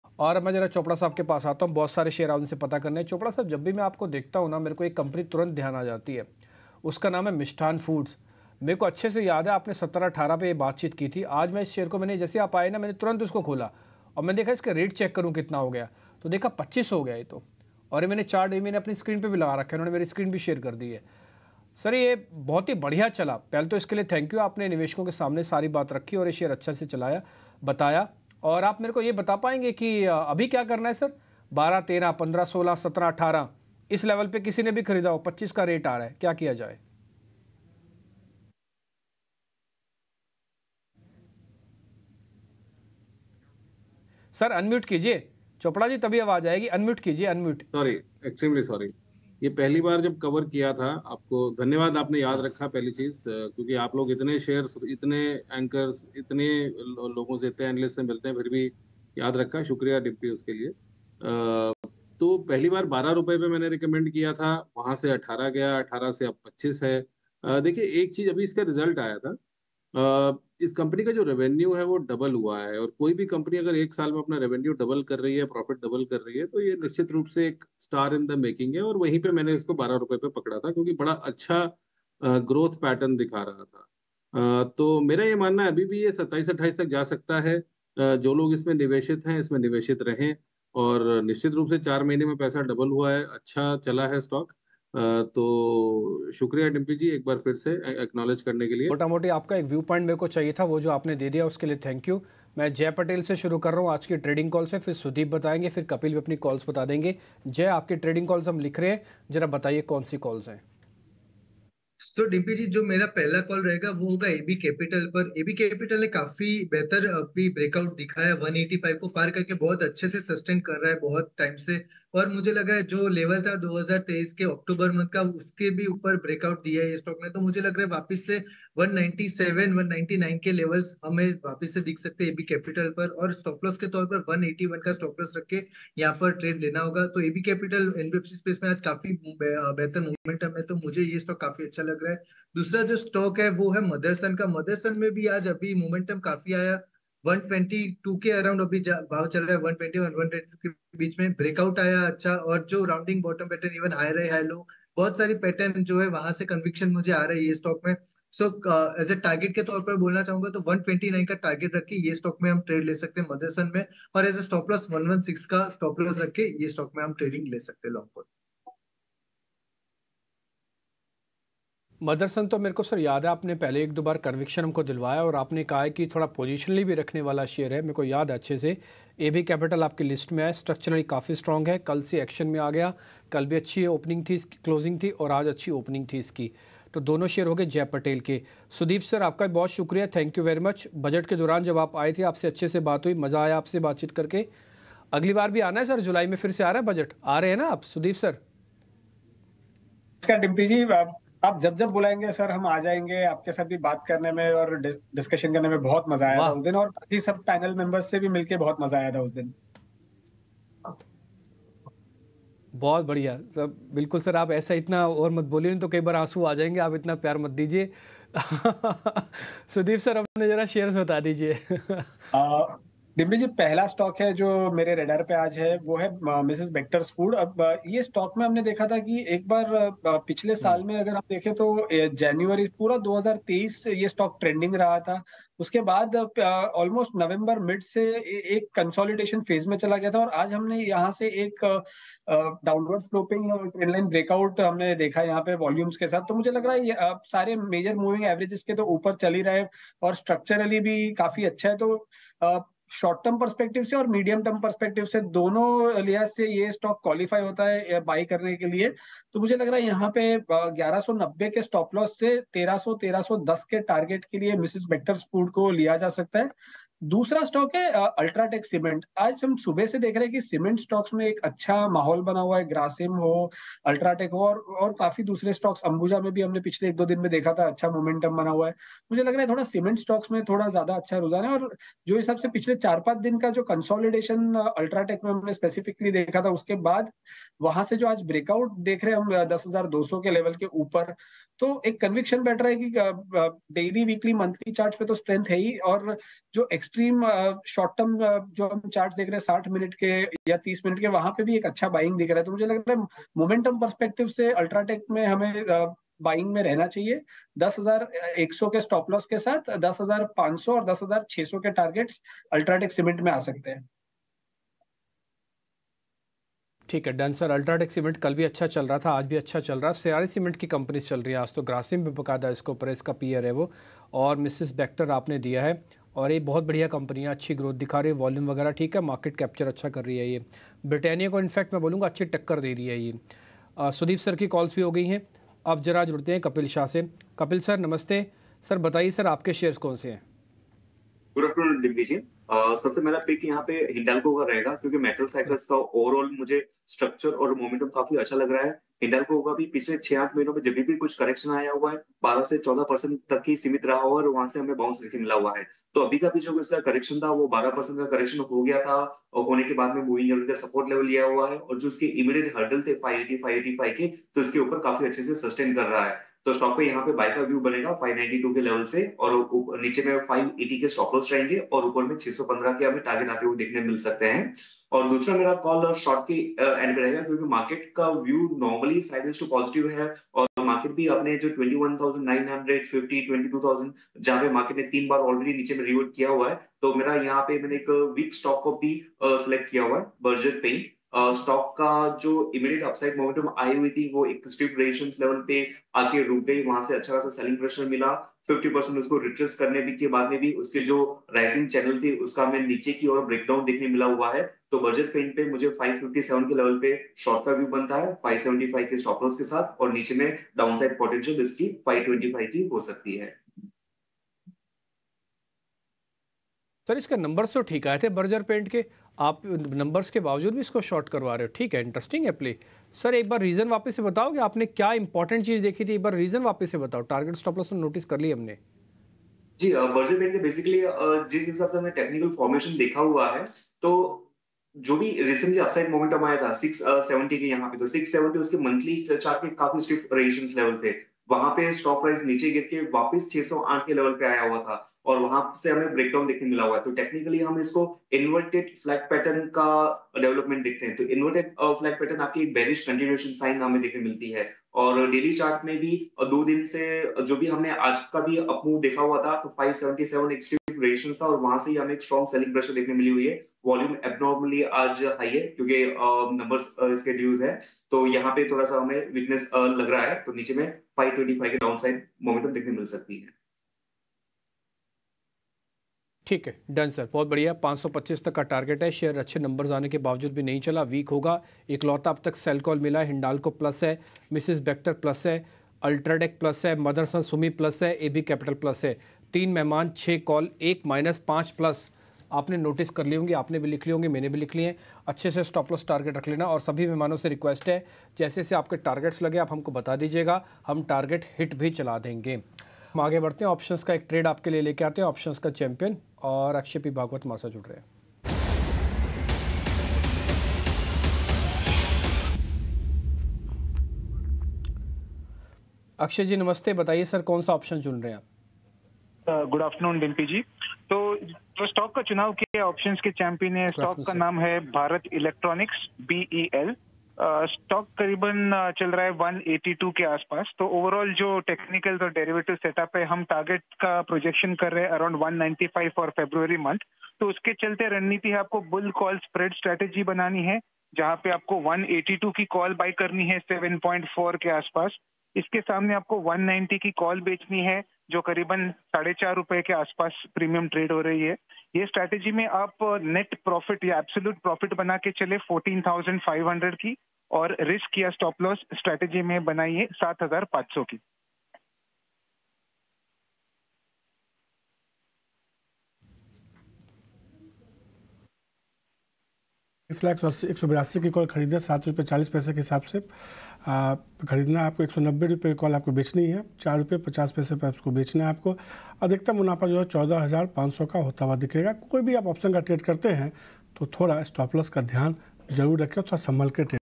Stocks to Buy Now : Experts से जानिए निवेशकों के लिए कहां है निवेश का मौका? | Mishtann Foods
Hindalco , Mishtann Foods, Aditya Birla Capital Share में जानें निवेश को लेकर क्या है Experts की राय। जानें किन levels पर करनी चाहिए आपको खरीदारी और क्या है Target Price और जानिए किस Strategy के साथ इस Stock में पैसा कमाया जा सकता है और अगर आप घाटे में है तो जरुर जाने की आप अपना घाटा कैसे काम कर सकते हैं। देखें वीडियो।